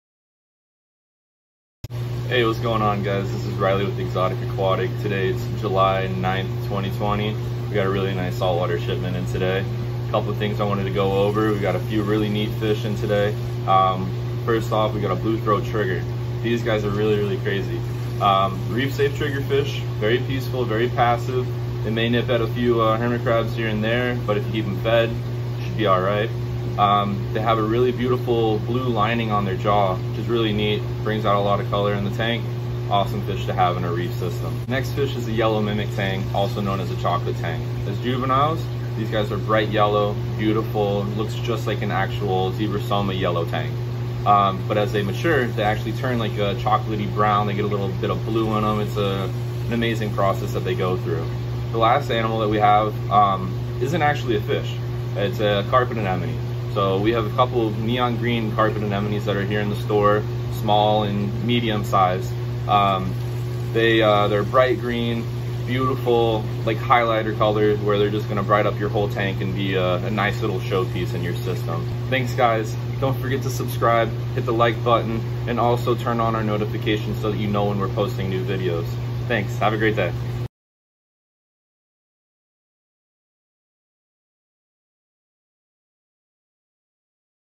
Weekend Arrivals! 07/10/2020
Riley presents some of our newest saltwater arrivals, including: Blue Throat Triggerfish, Mimic Tang and Carpet Anemones. Come in and see these and many more new animals!